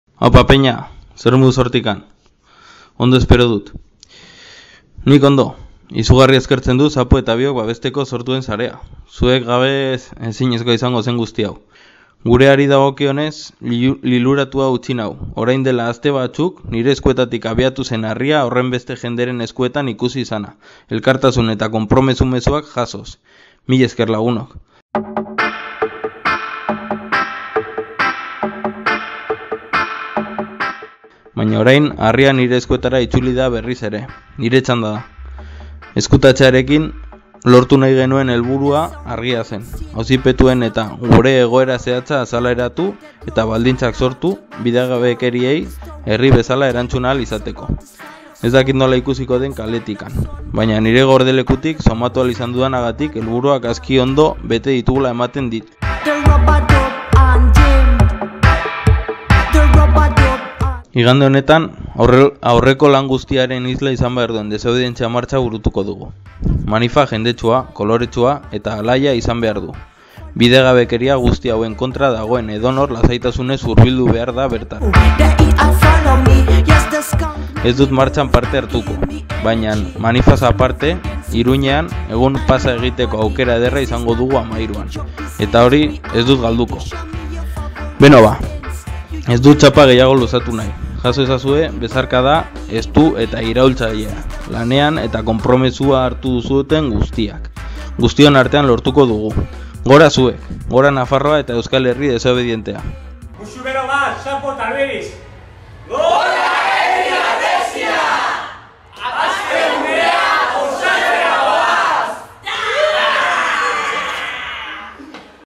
HerriHarresia Luis Goñi: "Nere txanda da! 13koa ez dut galduko!"
Luis Goñi mezu berri bat helarazi du euskal gizateari esanez datorren igandean Iruñean izango dela.